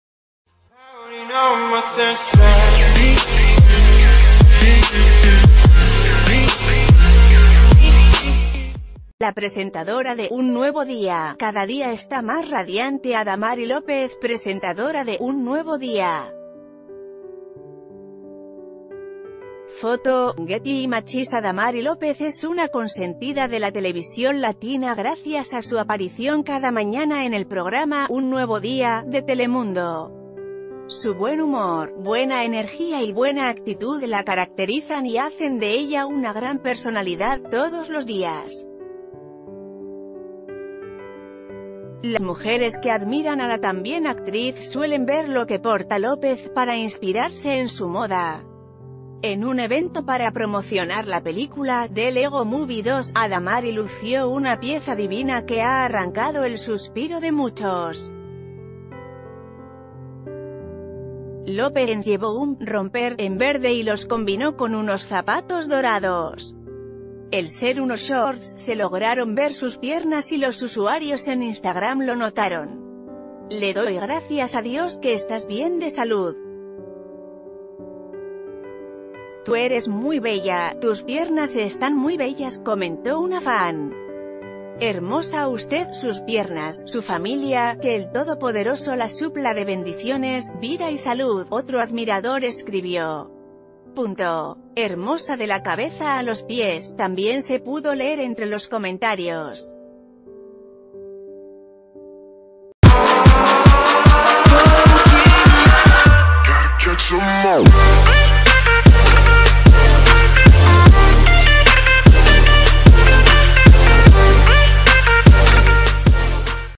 El sexy ‘romper’ de Adamari López que deja al descubierto sus piernas y arranca suspiros en Insta...
El sexy ‘romper’ de Adamari López que deja al descubierto sus piernas y arranca suspiros en Instagram
La presentadora de "Un nuevo día" cada día está más radiante Adamari López, presentadora de "Un nuevo día". Foto: Getty Images Adamari López es una consentida de la televisión latina gracias a su aparición cada mañana en el programa “Un nuevo día” de Telemundo. Su buen humor, buena energía y buena a...